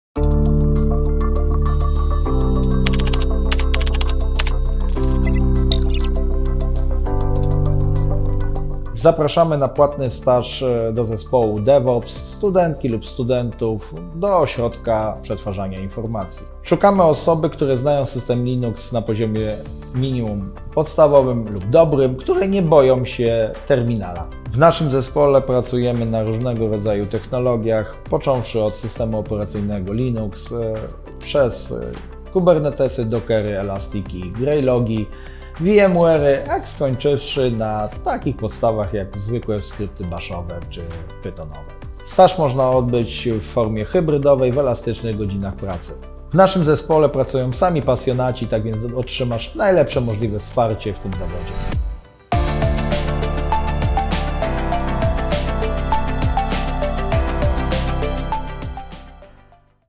KomITywa – ścieżka DevOps w płatnym programie stażowym OPIPIB!
Zgłoś się do płatnego programu stażowego KomITywa! Jeśli interesujesz się nowymi technologiami, a swoją drogę zawodową wiążesz z szeroko pojętym IT, to w OPI pokażemy ci, jak ten świat wygląda!
Ścieżka DevOps to jedna z propozycji stażu, na którą możesz aplikować. O tym, jakie czekają na ciebie wyzwania i zadania mówi Andrzej – tutaj w podwójnej roli – lidera Zespołu DevOps oraz opiekuna stażystów.
KomITywa to więcej niż świetny start! To co najmniej 60 angażujących i produktywnych dni, które spędzisz w miejscu, gdzie się pracuje zwinnie i z pasją.